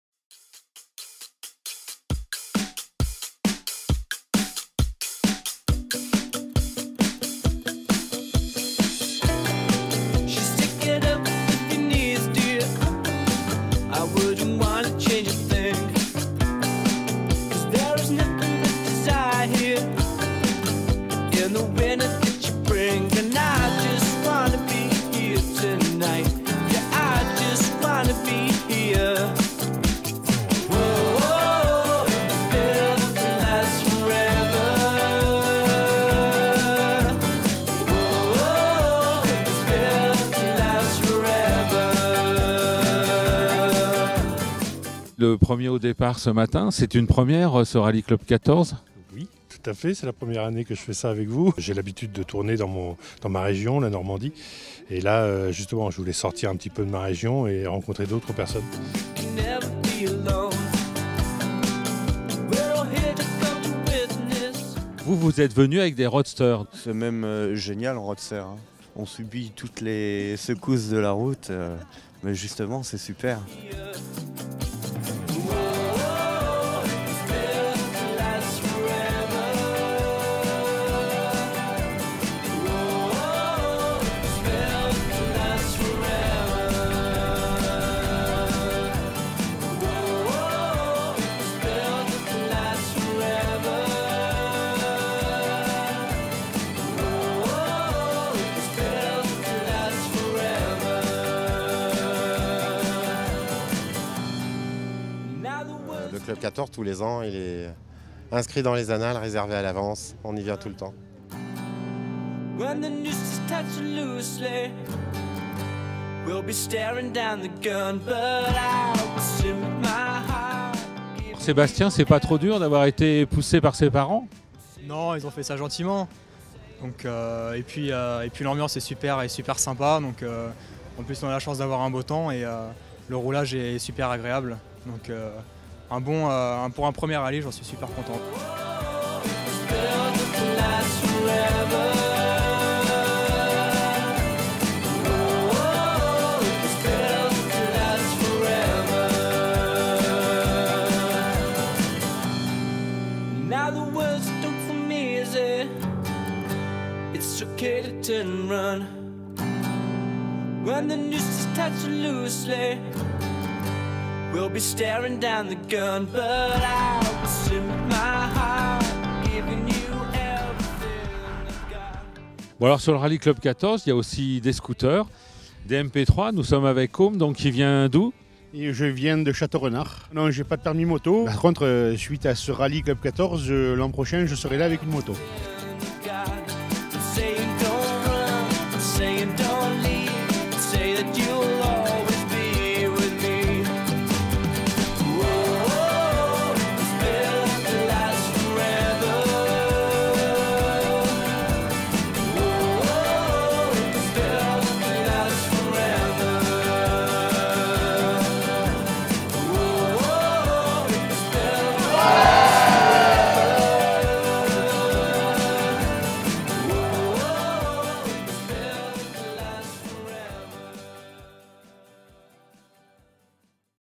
Rallye Club 14 édition 2013 - Alsace-Lorraine
Pour sa 29ème édition, le Rallye Club 14 s'est déroulé en Alsace-Lorraine.
Lors du weekend de l'Ascension (8 au 12 mai 2013), les participants ont pu découvrir ces régions lors de 3 journées de balade.